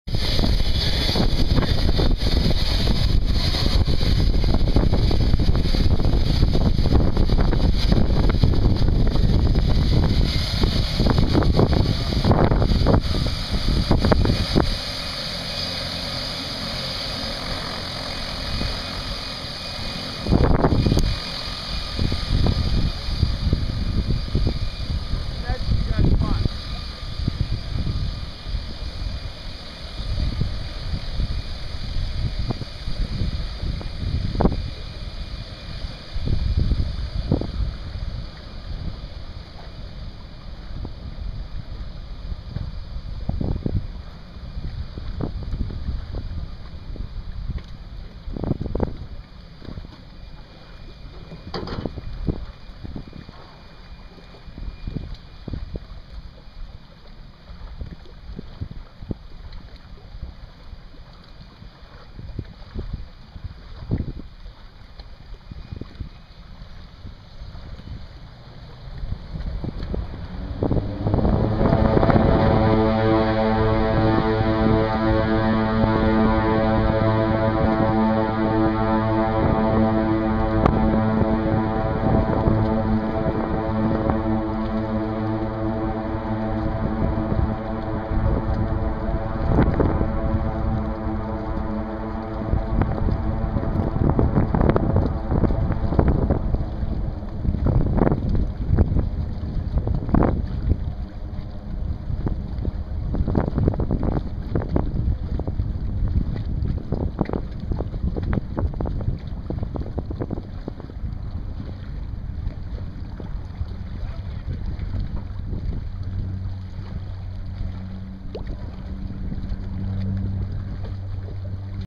Turbo Otter leaving Abbey Lake, ON
Hawk Air's turbo de Havilland Otter departing Abbey Lake (ON) outpost after mid-week camp check. June 2012